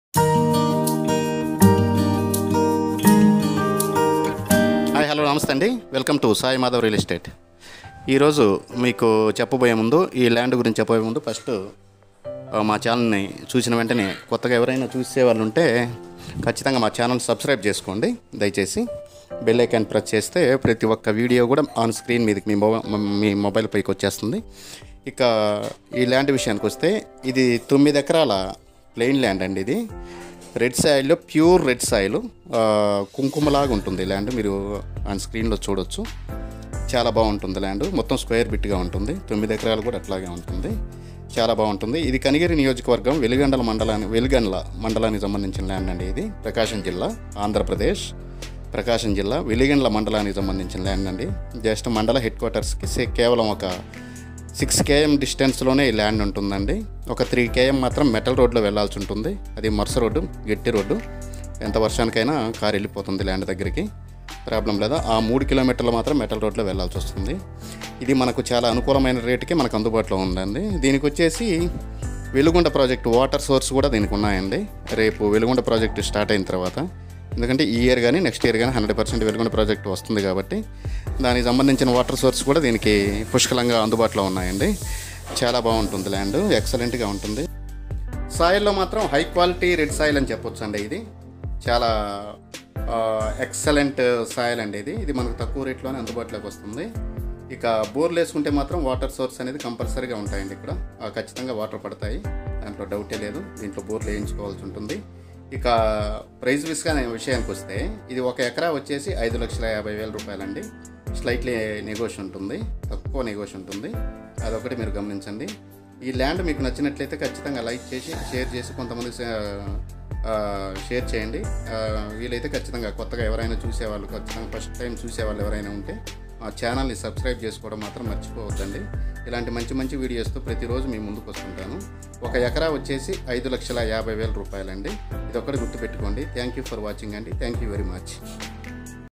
9 ACRES OF RED SOIL LAND FOR SALE.. PRAKASAM DT. VELIGANDLA. (AP). PH. NO. 8464955677.LAND:ID:228
9 acres of red soil land//3 km metal road from thar road//35 km from kanigiri //120 km from ongole //250 km from Vijayawada //450 km from east and west godavari //360 km from hyd //380 km from chennai //380 km from bengalore //ph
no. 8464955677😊